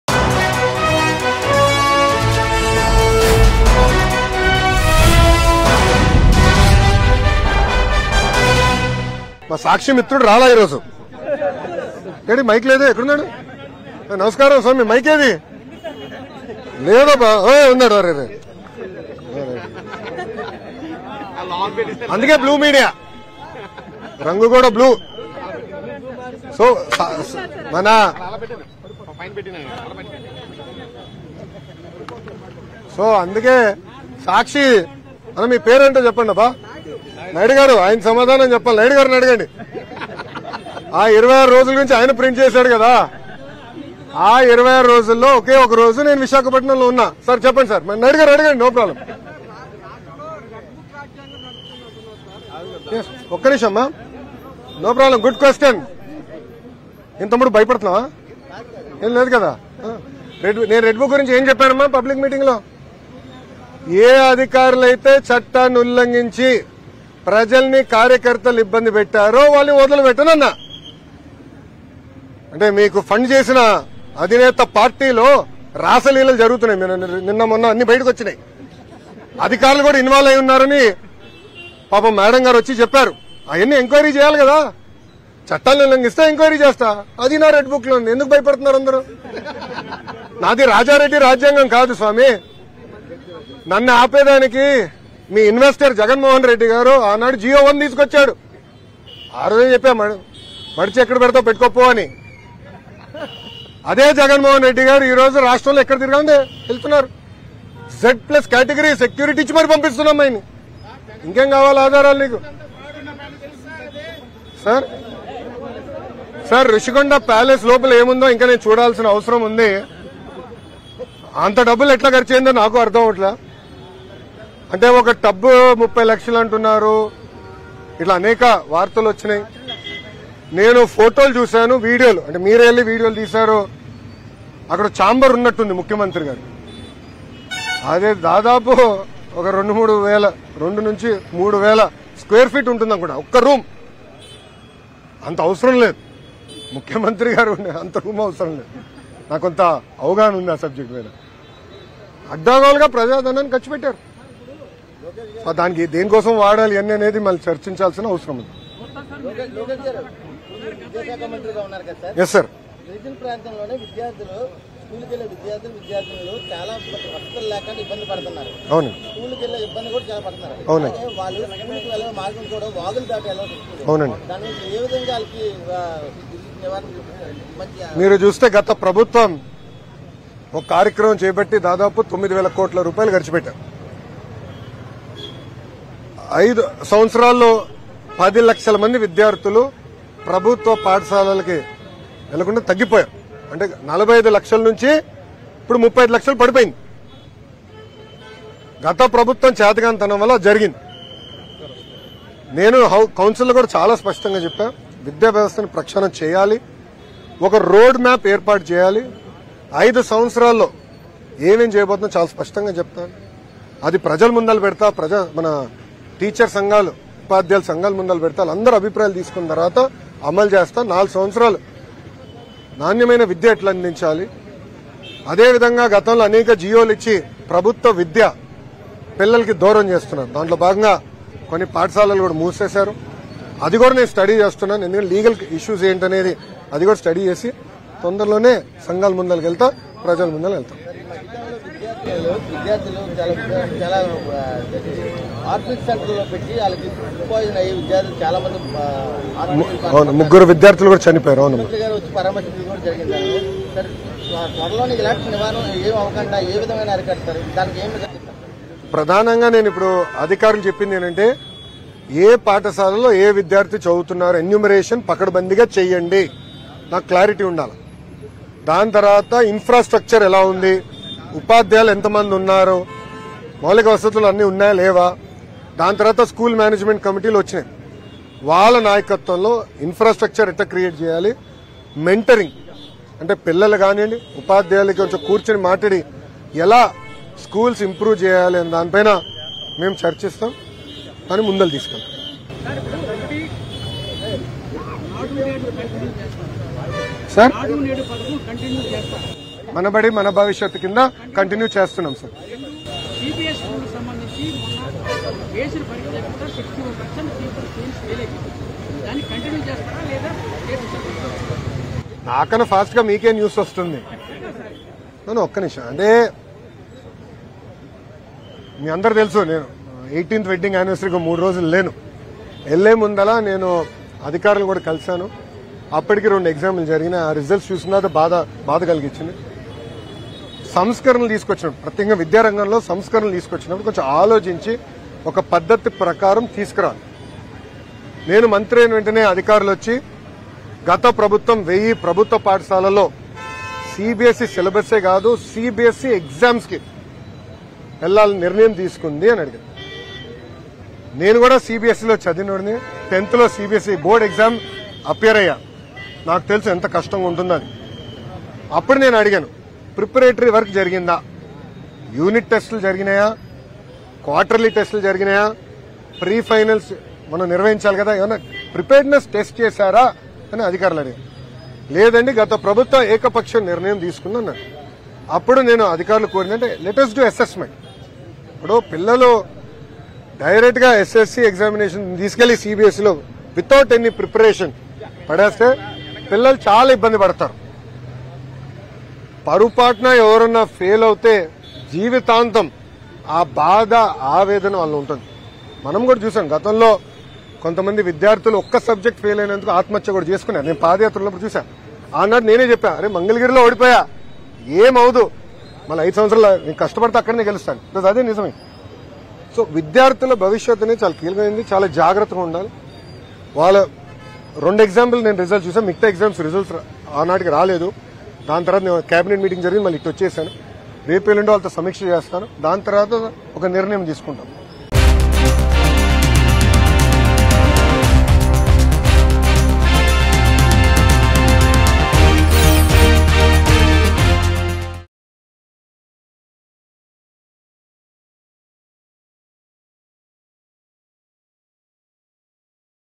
Minister Nara Lokesh Satirical Comments On YS Jagan | TDP Vs YCP | AP Politics | Mango News
Minister Nara Lokesh Satirical Comments On YS Jagan. He alleges that some government schools were closed during the YCP tenure.